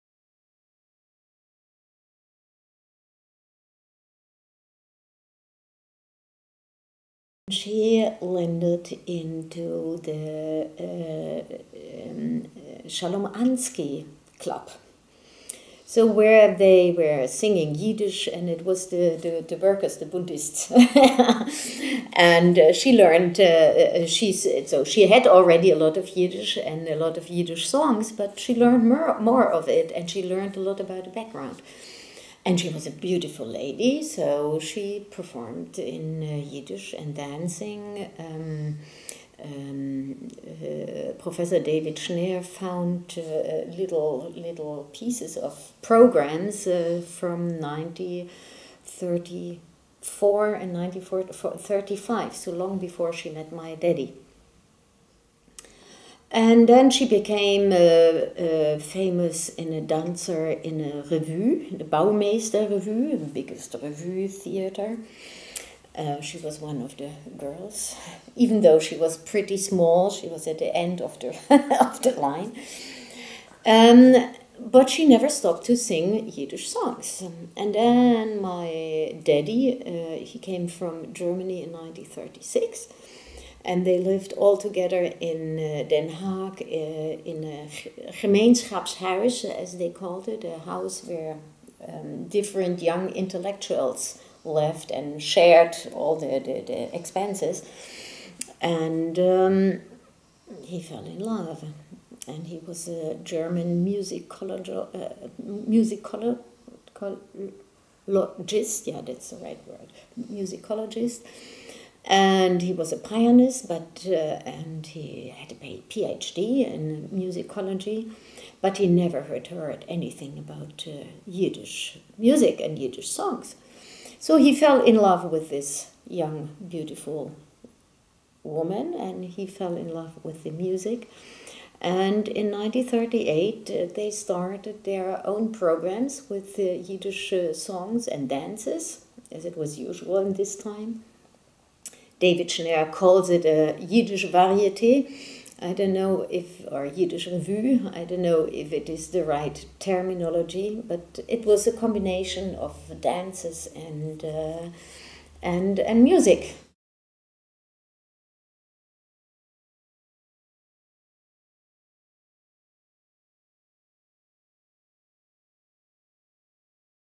The Start of Renowned Yiddish Performer Lin Jaldati's Career in 1930s Berlin
Jalda Rebling—Renewal Cantor and Yiddish performer—remembers how her parents who were both Holocaust survivors first met and worked together to help her mother, the renowned Lin Jaldati, begin a career in Yiddish performance in Berlin in 1938.